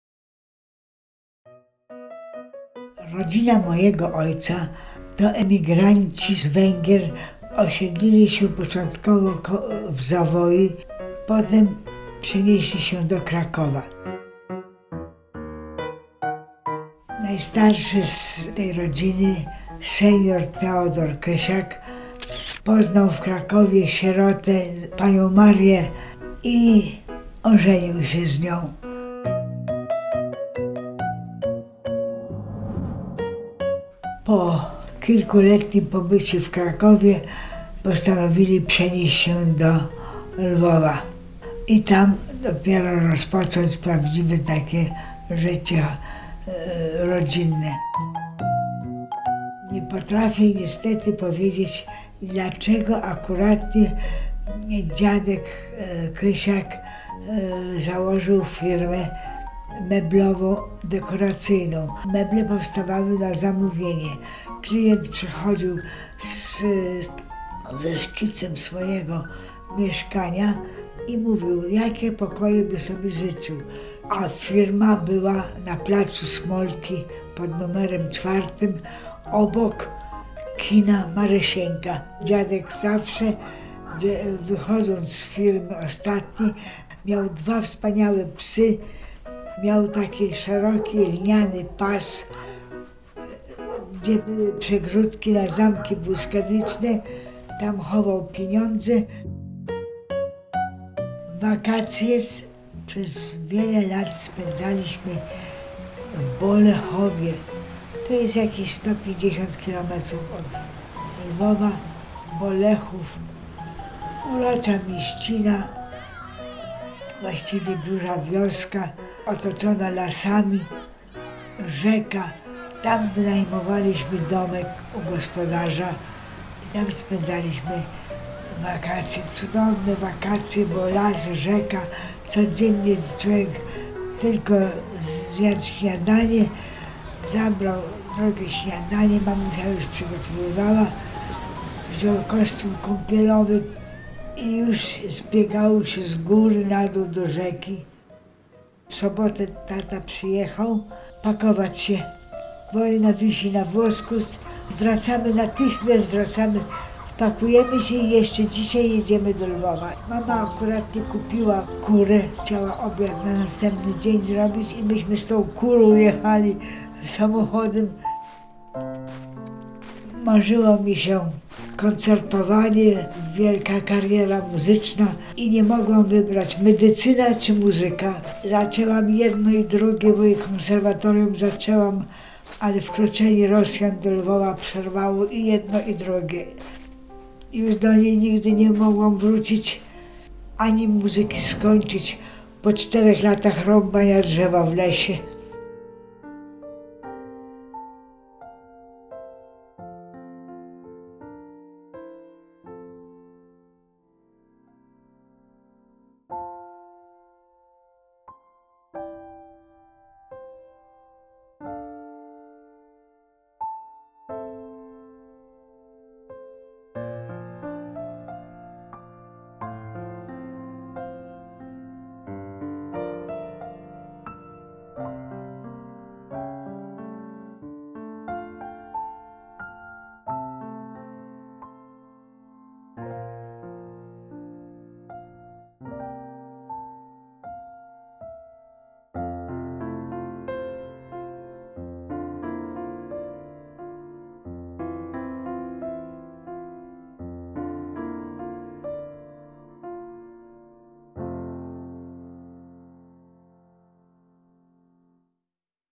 „Danusia ze Lwowa. Wspomnienia Danuty Kominiak” – Historia Mówiona Dzieciom
Historia opisana w książeczce to wciąż żywe wspomnienia wydarzeń sprzed wielu, wielu lat. Danuta Kominiak opowiada o swoim dzieciństwie jak Lisa z „Dzieci z Bullerbyn”. Z tą różnicą, że wszystko zdarzyło się naprawdę… Opowieść została nagrana i trafiła do Archiwum Historii Mówionej Domu Spotkań z Historią i Ośrodka KARTA. W tej wielkiej „bibliotece” jest już kilka tysięcy wywiadów i ciągle przybywają nowe. Relacja Danuty Kominiak otwiera serię pięknie ilustrowanych książeczek „Historia Mówiona Dzieciom”.

Pomysł i opracowanie książki: Magdalena Czerwiec-Pichlińska

Ilustracje: Krzysztof Zięba

Współpraca: Magda Szymańska

Konsultacja merytoryczna: Sebastian Pichliński

Montaż i animacja: Mikołaj Brożek

Wydawca: Dom Spotkań z Historią

Warszawa, 2018